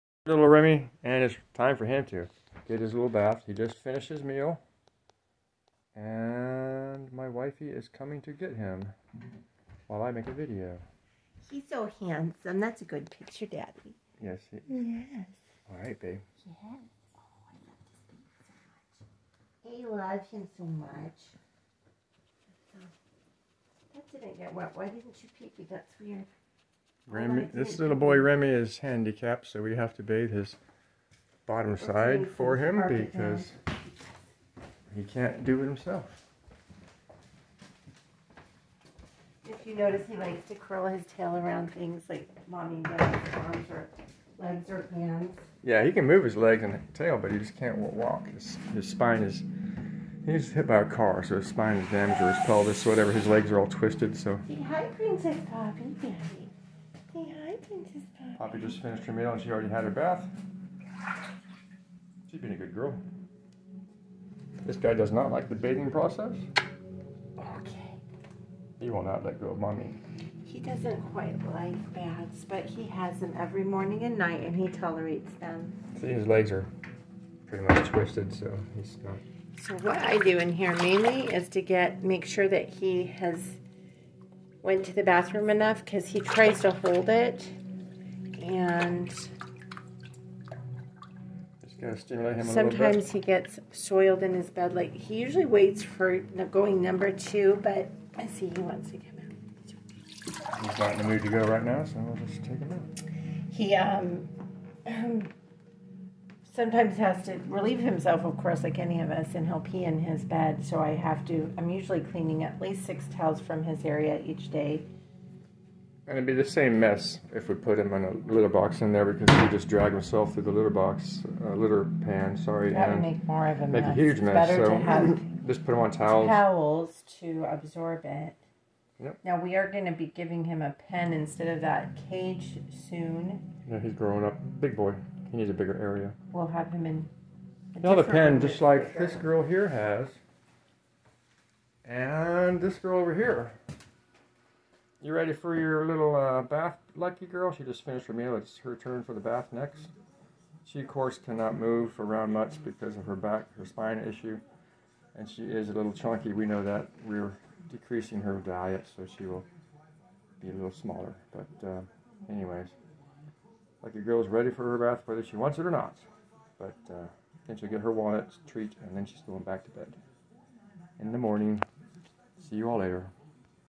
Opossum morning. Remi, Poppy and Lucky Girl in the morning.
Remi gets a bath while Poppy grooms herself after her meal and Lucky Girl waits for her bath.